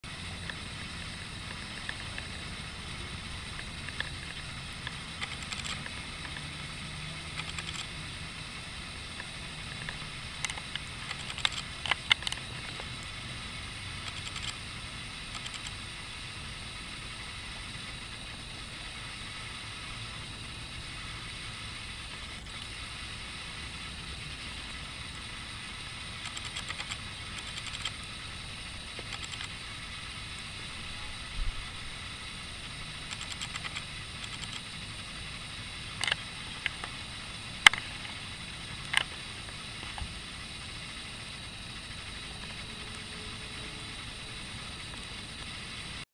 Meniscus midge, in hillside seep
The larva of a meniscus midge (Diptera: Dixidae) wends its way over the watery substrate of a hillside seep or spring in Winneshiek County, Iowa, March 30, 2018